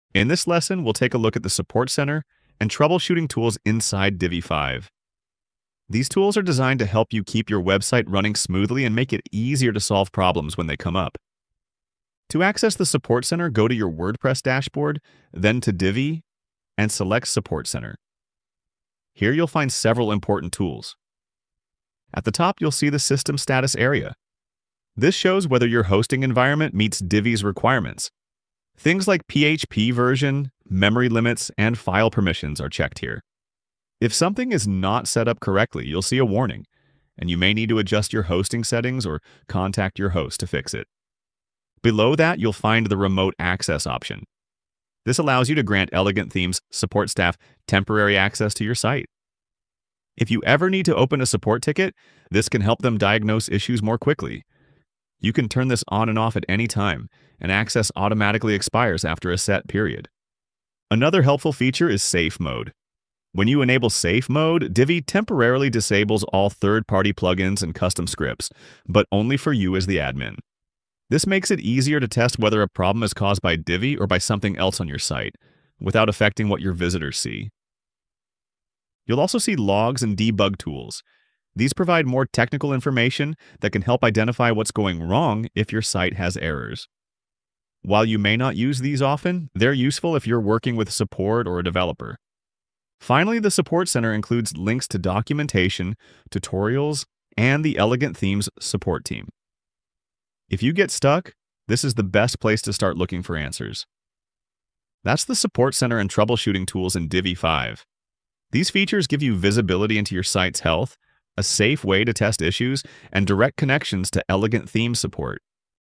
Divi 5 Support Center | Tools to Fix & Optimize Your Website
Discover the Support Center in Divi 5 and how to use its troubleshooting tools to identify and fix common issues quickly.